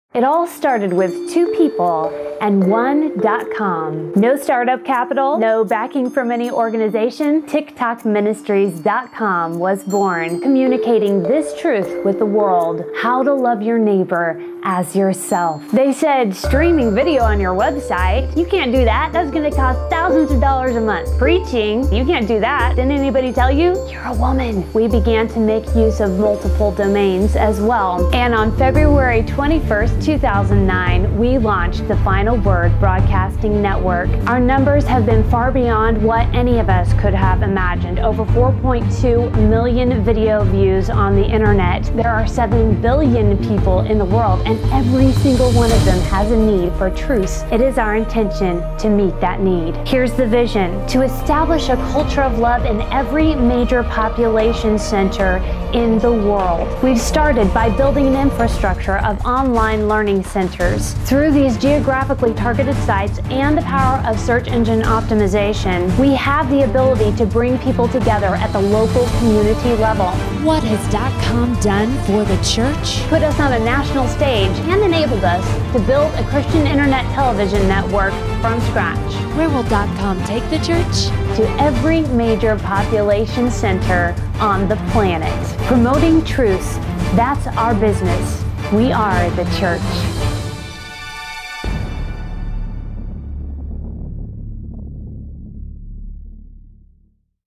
The Church, The Bride, The Vision: Establishing A Culture Of Love
While the Bride anxiously awaits the return of her King, God's plans and vision for the Church begin to come into view. Find out what steps Tic Toc Ministries is taking to move forward with this vision, and how you can be a part of God's end-times mission: to establish a culture of love in every major population center in the world.

Please watch this important message: "Mayday Prophecy For Pastors: You Are Being Judged "